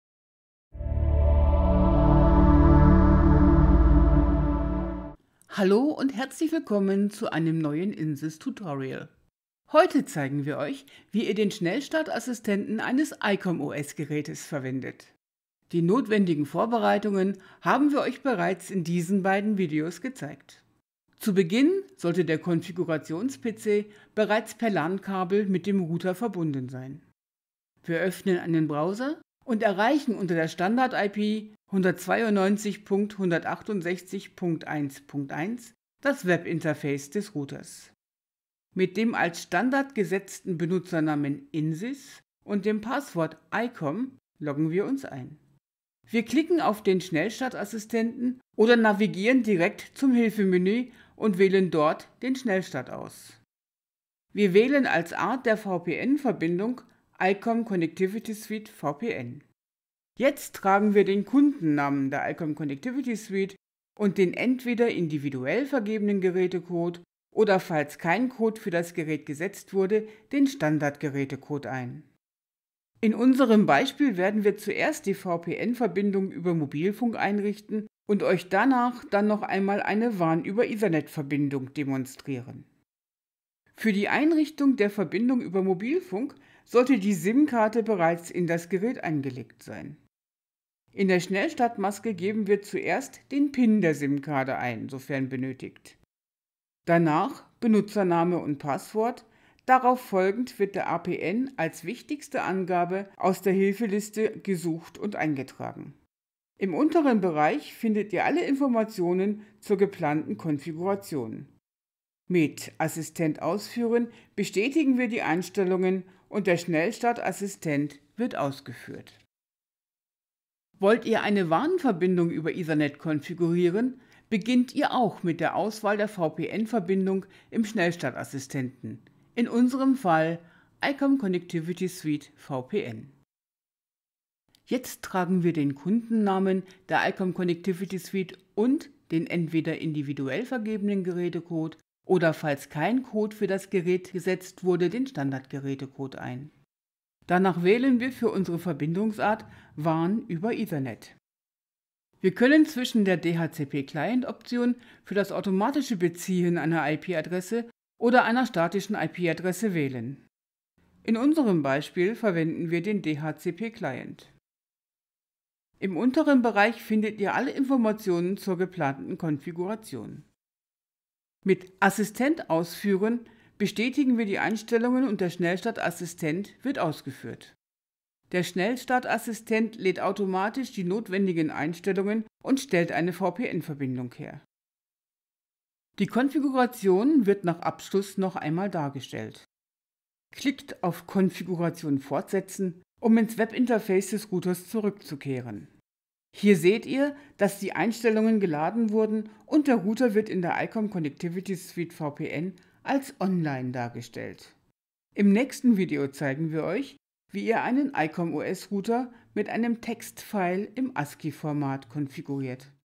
Aufbau einer VPN Verbindung mit einem icom OS Gerät zur icom Connectivity Suite – VPN
In diesem Video zeigen wir Ihnen, wie der Schnellstart bei icom OS Routern durchgeführt wird.